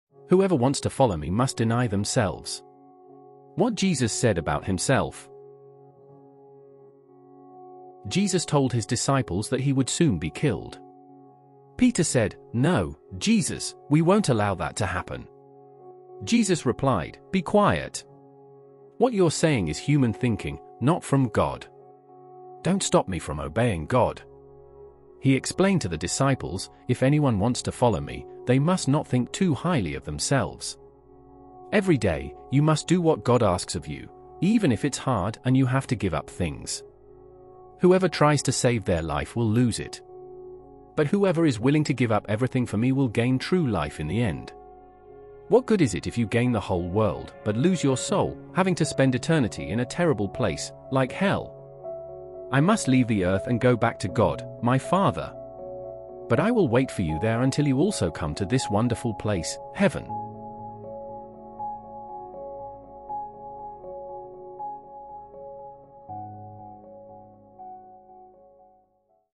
Whoever wants to follow me, must deny himself
Bibletext (KJV) Luke 9:
21 And he straitly charged them, and commanded them to tell no man that thing;

22 Saying, The Son of man must suffer many things, and be rejected of the elders and chief priests and scribes, and be slain, and be raised the third day.

24 For whosoever will save his life shall lose it: but whosoever will lose his life for my sake, the same shall save it.

25 For what is a man advantaged, if he gain the whole world, and lose himself, or be cast away?

26 For whosoever shall be ashamed of me and of my words, of him shall the Son of man be ashamed, when he shall come in his own glory, and in his Father's, and of the holy angels.

27 But I tell you of a truth, there be some standing here, which shall not taste of death, till they see the kingdom of God.